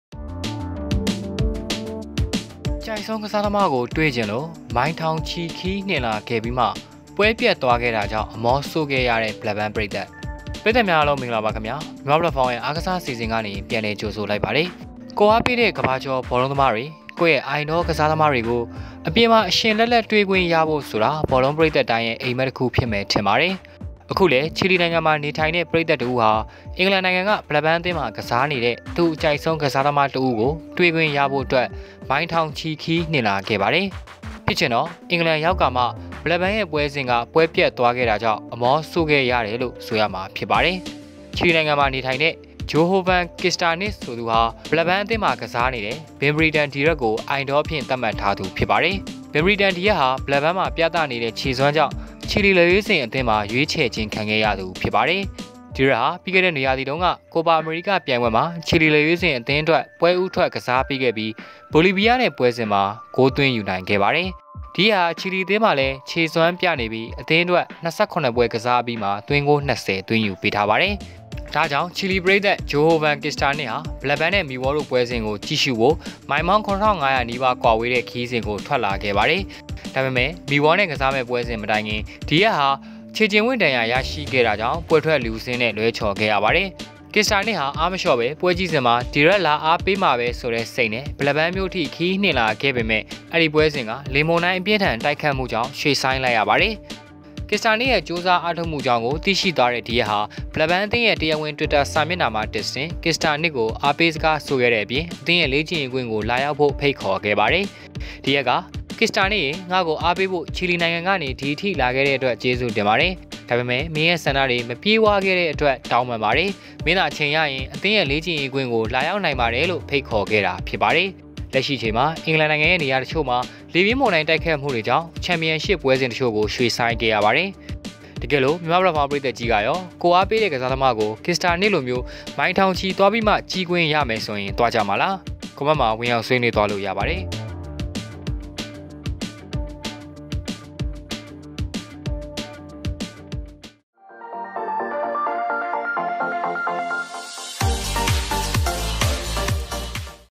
တွေ့ချင်လို့မိုင်ထောင်ချီခရီးနှင်လာပြီးမှ ပွဲပျက်သွားခဲ့တာကြောင့် အမောဆို့ခဲ့ရတဲ့ ဘလက်ဘန်းပရိသတ်
အကြိုက်ဆုံးကစားသမားကို တွေ့ချင်လို့မိုင်ထောင်ချီခရီးနှင်လာပြီးမှ ပွဲပျက်သွားခဲ့တာကြောင့် အမောဆို့ခဲ့ရတဲ့ ဘလက်ဘန်းပရိသတ်